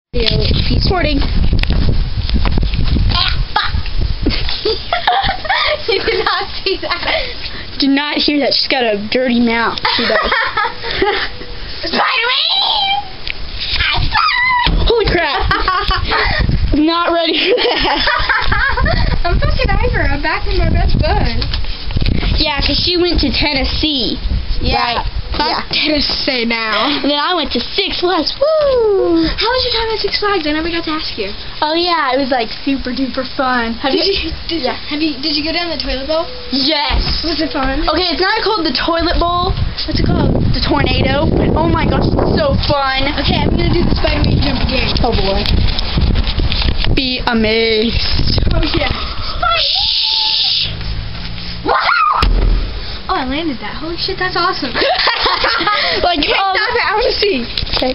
Spiderman! Jumps
Rachel and Aubrey are in the gazebo at the funeral home, when Rachel starts to be Spiderman! Be amazed at what is Rachel the spider~ Whoosh.